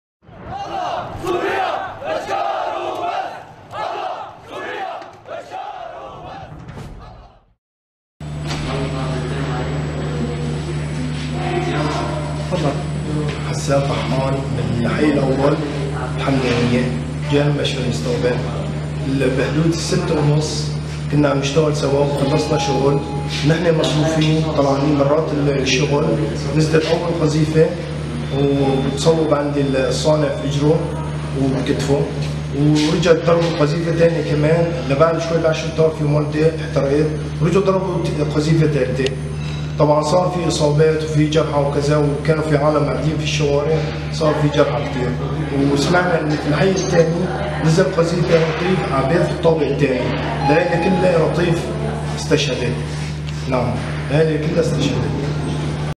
Сирия. Война в Сирии.Террористы убивают детей в крупнейшем городе Сирии
Сирия. Война в Сирии. Syria. The war in Syria.Terrorists kill children in Syria's largest city.

Идлибские банды продолжают терроризировать самый крупный сирийский город Алеппо, нанося удары по густонаселённым жилым кварталам. Снова убиты дети...

Об этом в новом репортаже «Русской Весны» из Сирийской Арабской Республики.

В результате вражеского удара обстрела пострадали 10 человек, 4 из них погибли, 6 получили тяжёлые ранения.

Один из местных жителей рассказал «РВ» о смертоносном обстреле боевиков: Читайте больше на

Эти кадры не предназначены для насильственного сенсационного шокирующего действия ..